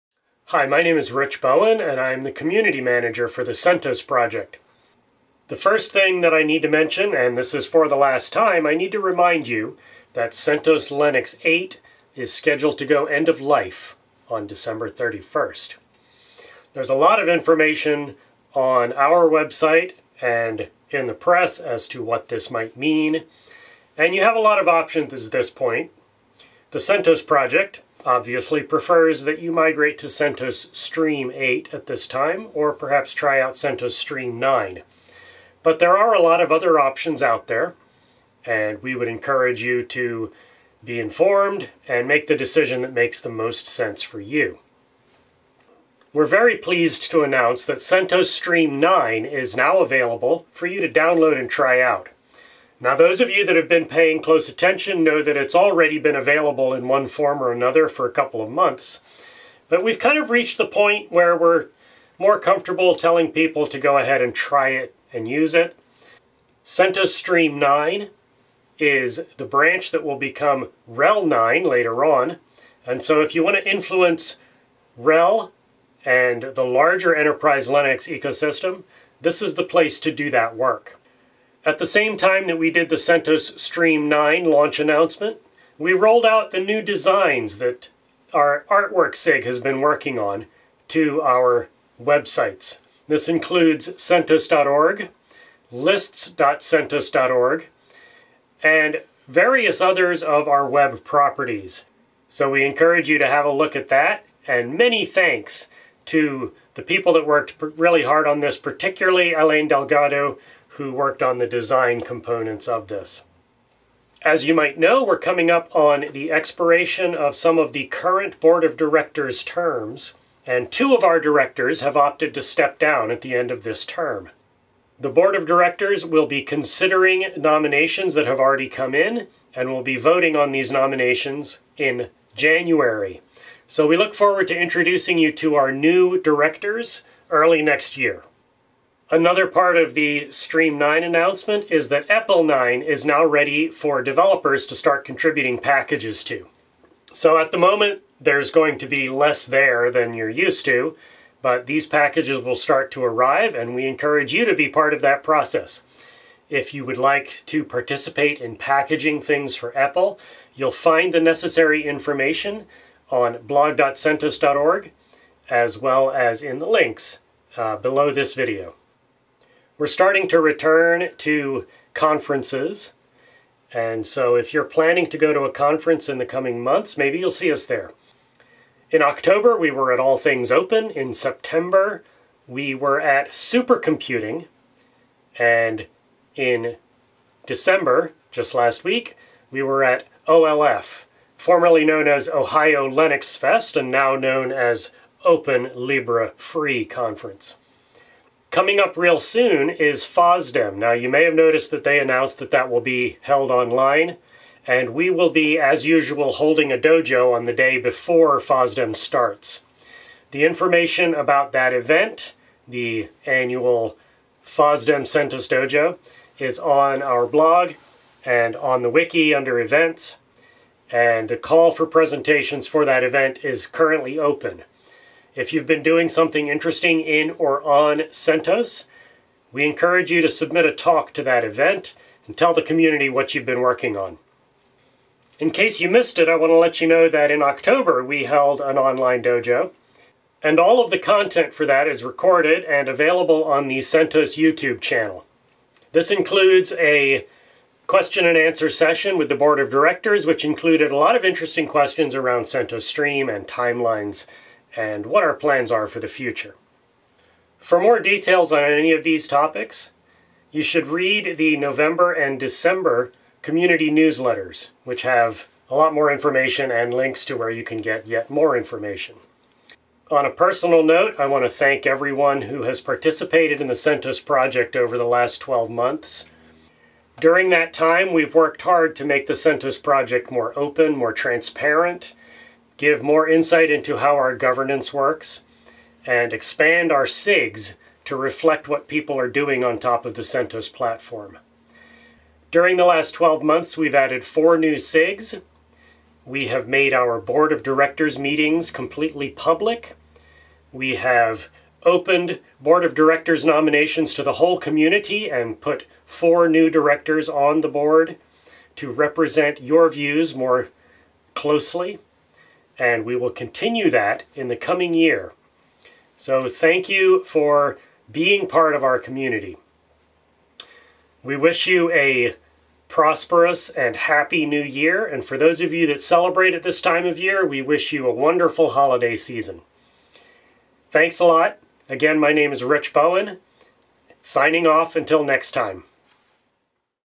CentOS News, December 2021
News summary from December, 2021

This video was recorded using OBS Studio, and edited in kdenlive. Additional overlay images were created using Krita.

00:00 Intro
00:04 CentOS Linux 8 EOL
00:45 CentOS Stream 9 launch
01:20 New website design!
01:54 Board of Directors election
02:18 EPEL 9
02:48 Events
03:55 October Dojo
04:17 In conclusion ...